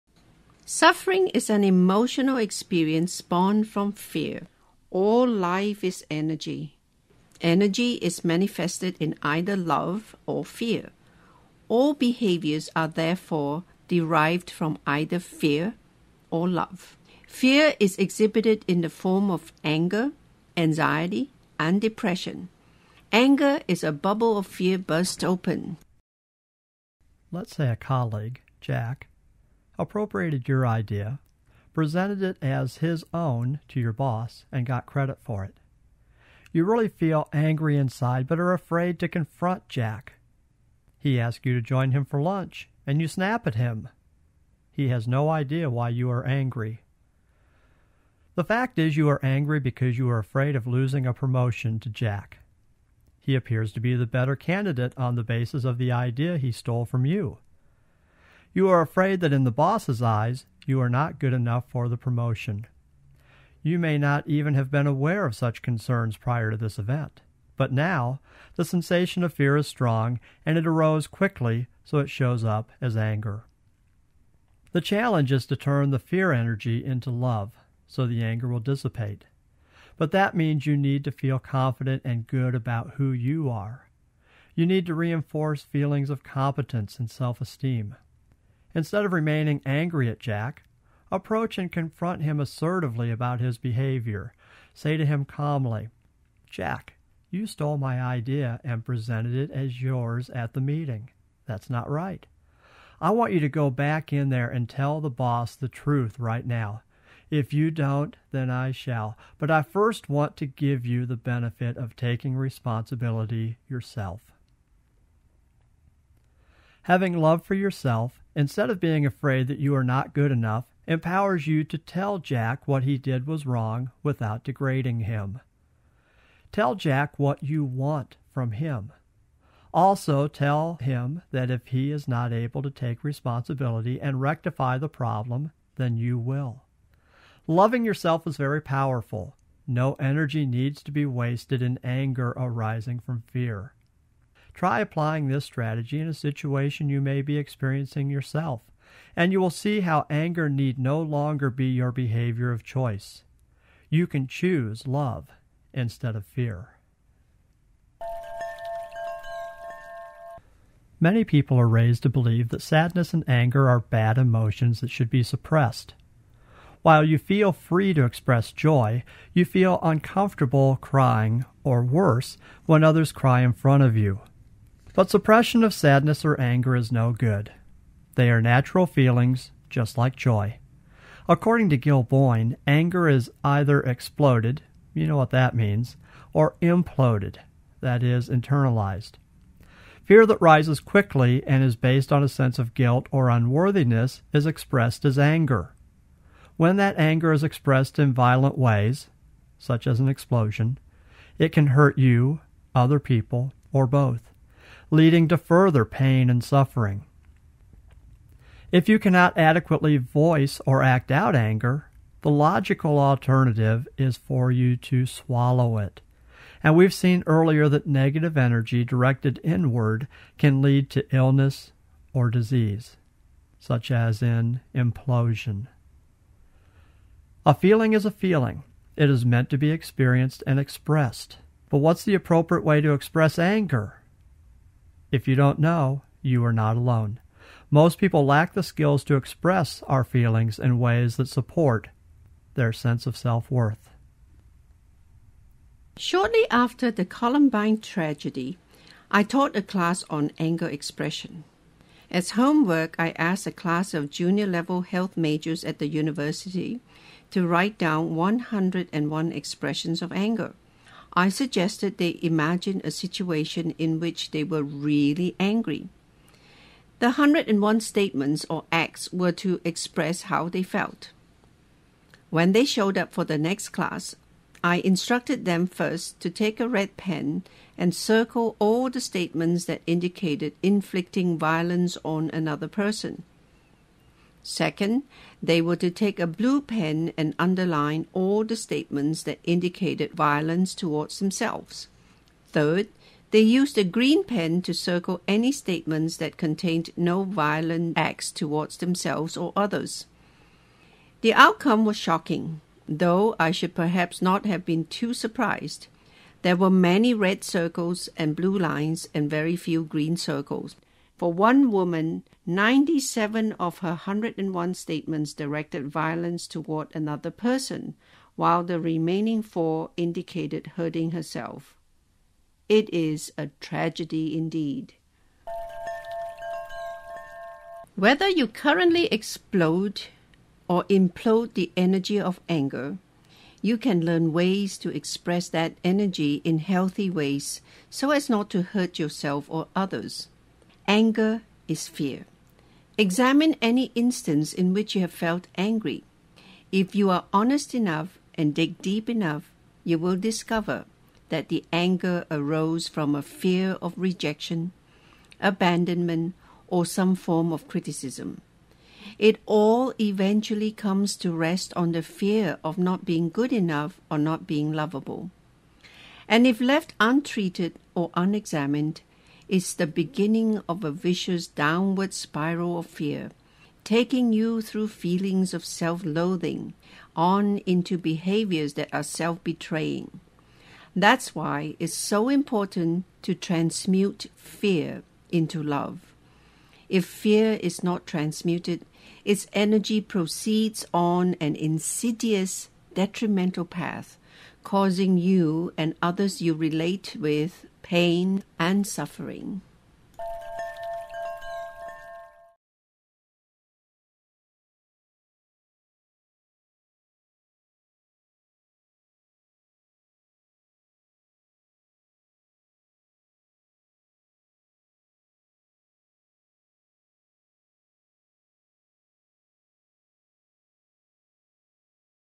Genesis of Suffering
The amount of suffering is directly related to either self-love or fear. Unexpressed (hidden) fear can have potentially devastating consequences beyond the immediate pain.